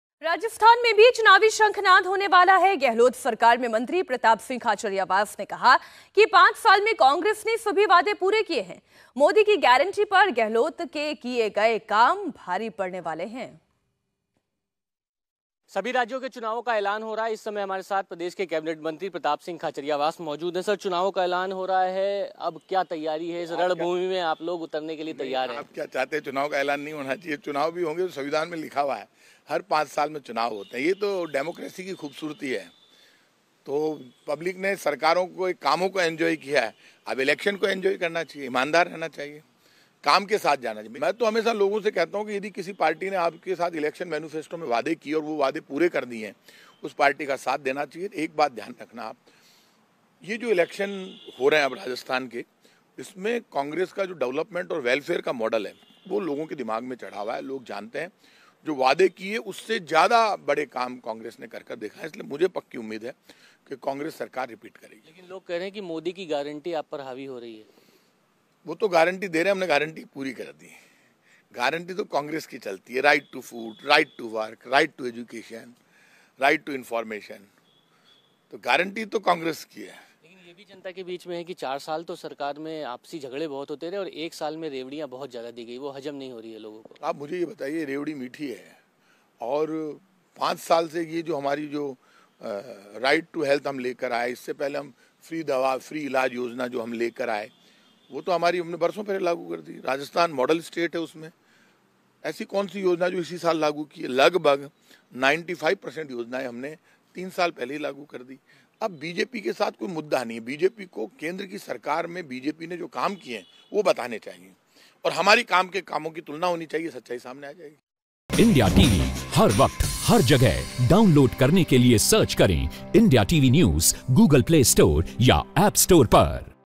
Rajasthan Assembly Poll: राजस्थान में सालों पुराना ट्रेंड बदलेगा..एक बार फिर आएगी Congress?
Rajasthan Assembly Election 2023: राजस्थान में भी चुनावी शंखनाद होने वाला है. गहलोत सरकार में मंत्री प्रताप सिंह खाचरियावास ने कहा कि 5 साल में कांग्रेस ने सभी वादे पूरे किए हैं.